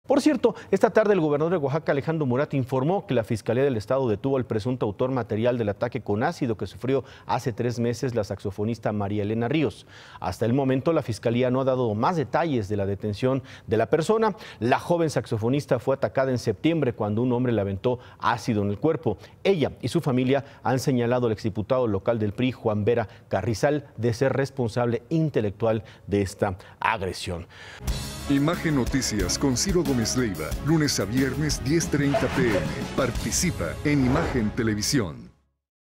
Detienen a quien roció de ácido a saxofonista | Noticias con Ciro Gómez Leyva
Fue detenido un hombre acusado de rociar con ácido a la saxofonista María Elena Ríos, así lo informó el gobernador de Oaxaca, Alejandro Murat.